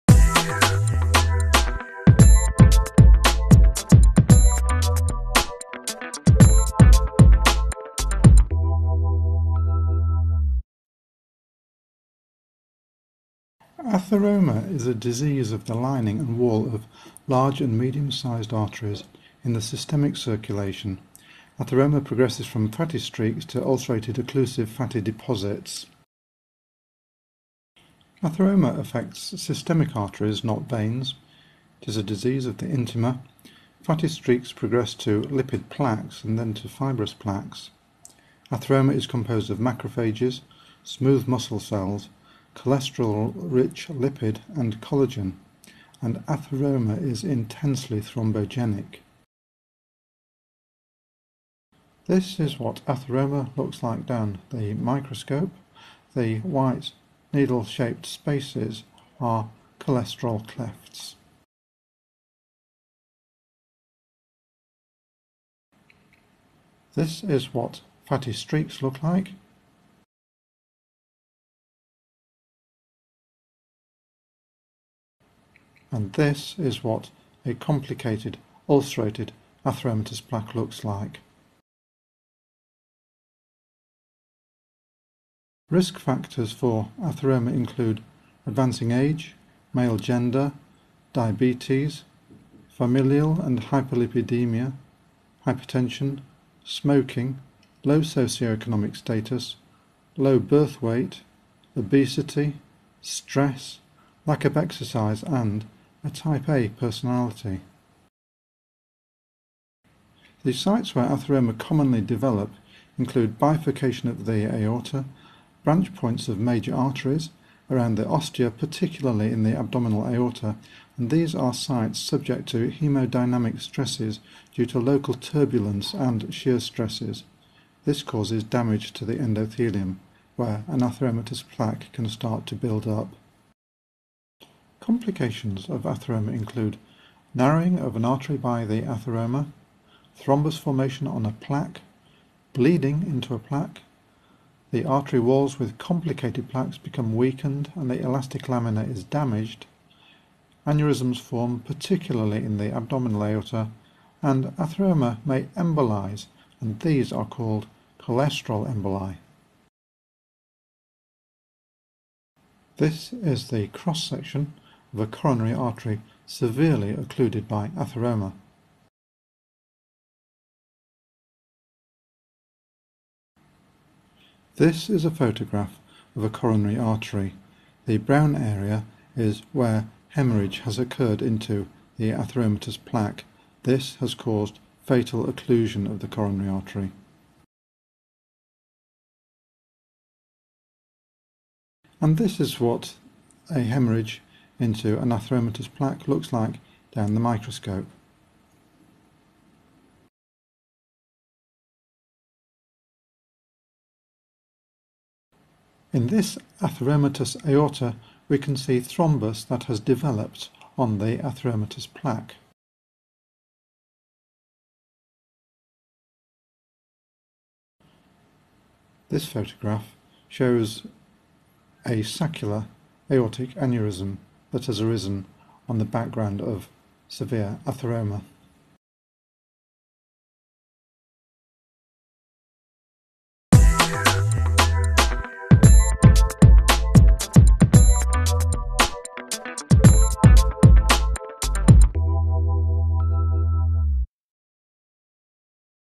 Atheroma - Pathology mini tutorial
The pathology and complications of atheroma
What is atheroma? 0:15
Description of evolution of atheroma 0:29
Microscopic appearance of atheroma 0:52
Fatty streak gross image 1:06
Complicated atheromatous plaque image 1:16
Risk factors for atheroma 1:26
Atheroma sites 1:50
Complications of atheroma 2:14
Coronary artery atheroma image 2:45
Bleeding into atheromatous plaque image 2:54
Thrombosis of atheroma image 3:28
Saccular aortic aneurysm image 3:37